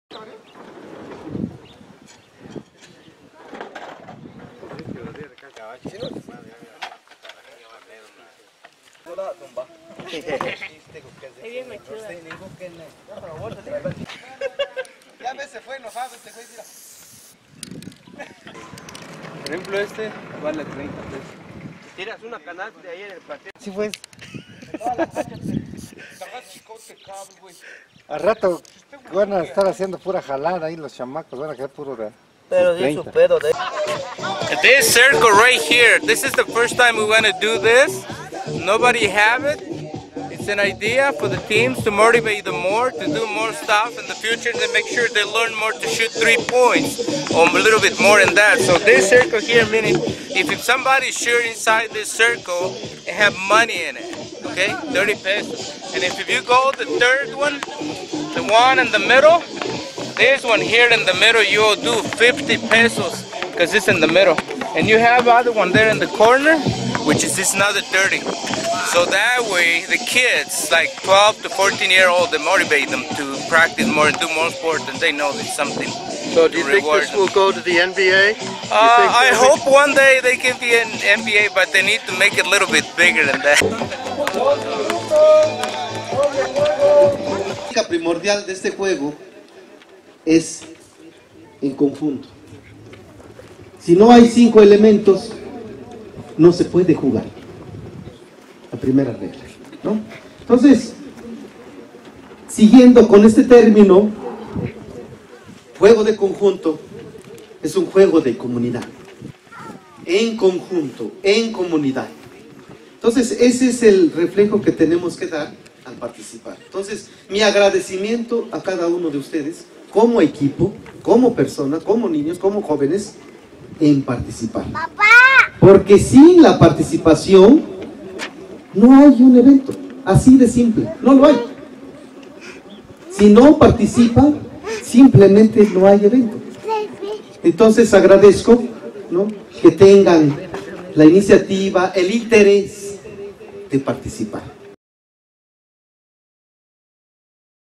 Hongos
The Hongos basketball team hosts the beginning of the season at their home court in Teotitlan del Valle.